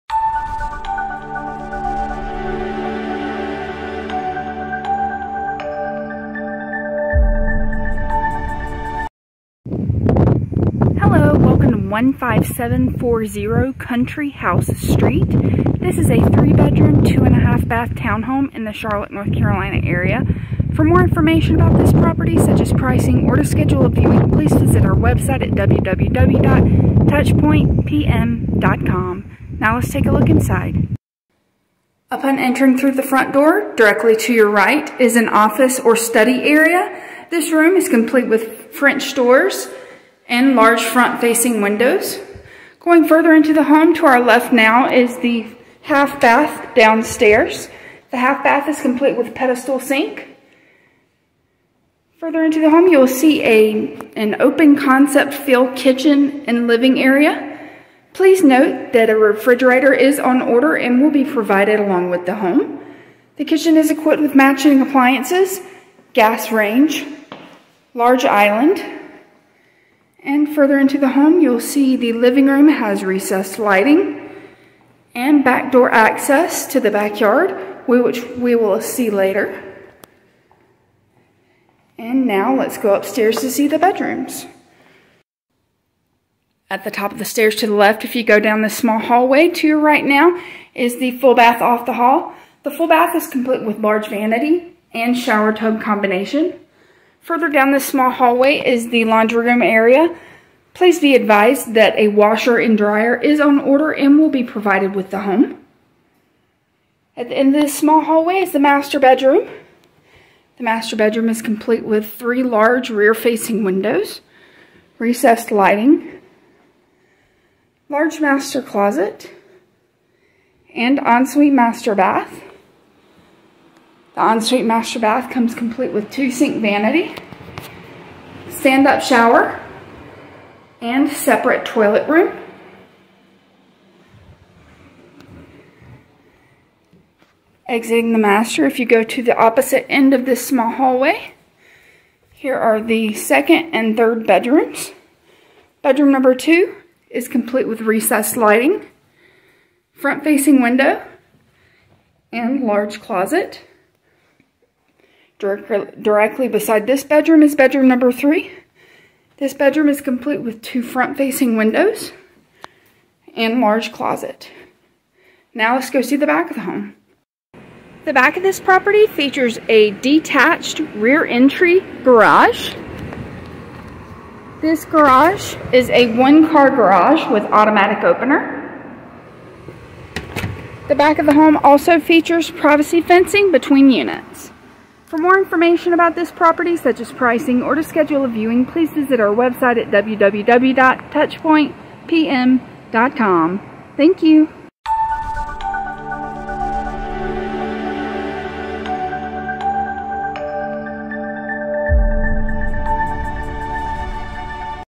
Charlotte Townhomes for Rent 3BR/2.5BA by Property Manager in Charlotte
This townhome features a lovely open-floor plan that connects the living area to the kitchen - perfect for entertaining! Also downstairs are a half bath and an office with French doors. Upstairs are the central bathroom, a laundry room, 2 secondary bedrooms, plus the master suite with an ensuite bath and a walk-in closet. Outside is a detached rear-entry 1-car garage for your parking needs.